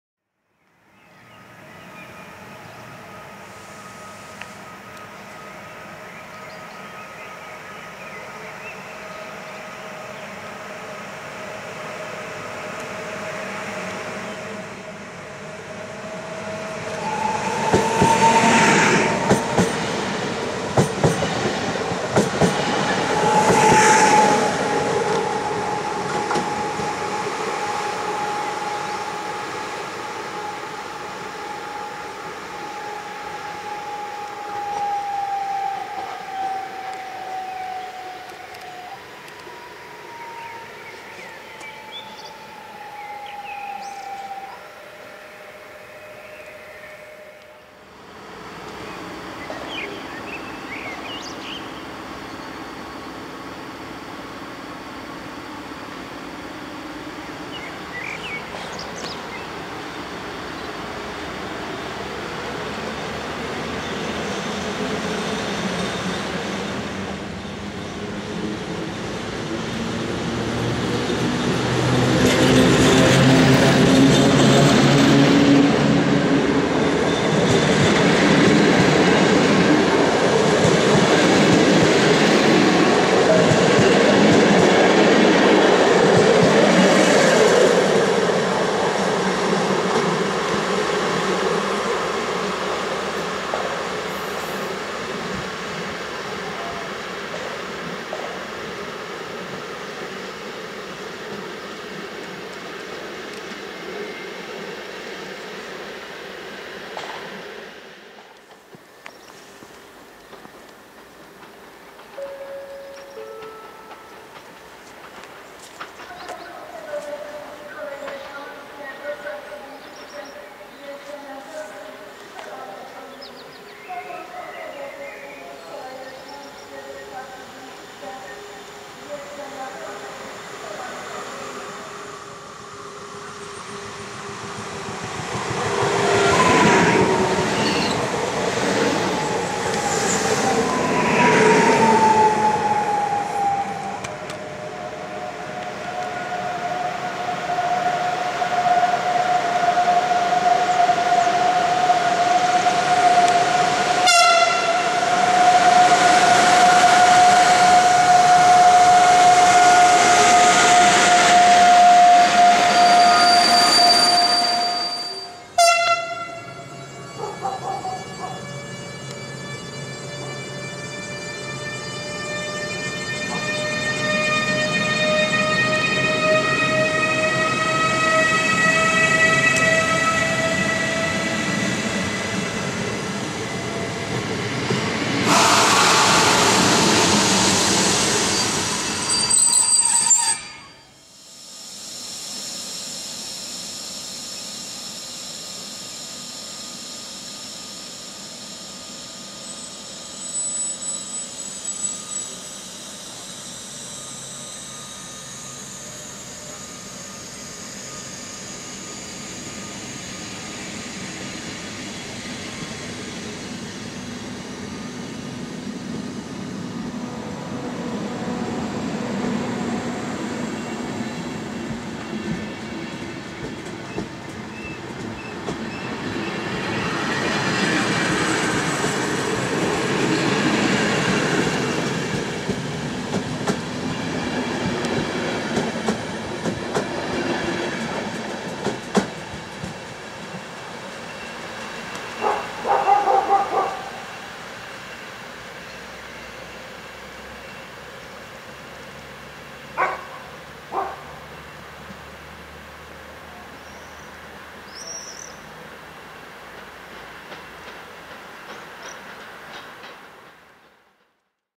(6.06.12) Dwa ELFy, Byk i Husarz w Tychach (+2xRp1 i zatrzymanie tuż po odjeździe)
Krótki podwieczorny wypad na tyski dworzec podczas którego sfilmowałem:
0:00 EN76-007 odjeżdżający jako pociąg osobowy KŚ-44023  Katowic do Wisły Głębce
0:47 ET22-1028 odjeżdżający z InterRegio "Skrzyczne" z Warszawy do Bielska
1:48 EN76-005 wjeżdżający jako KŚ-44024 z Wisły do Gliwic i pierwszy Husarz w barwach Polski wjeżdżający z Klimczokiem z Gdyni do Bielska (+Rp1)
2:44 Odjazd Klimczoka (+Rp1) i zatrzymanie składu tuż po odjeździe. Nie jestem pewien co do powodu zatrzymania, ale coś słyszałem o Radio STOPie. Po 5 minutach postoju skład ruszył w dalszą drogę.